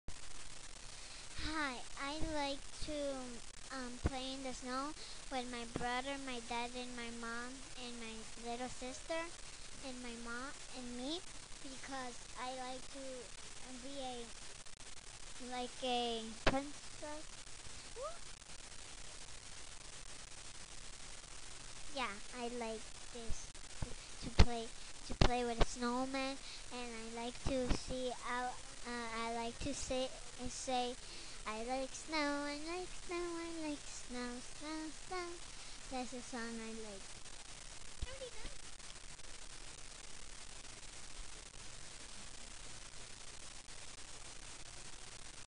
Snow EMH
First grader created a picture to show things she enjoys about the snow and recorded her thoughts about it.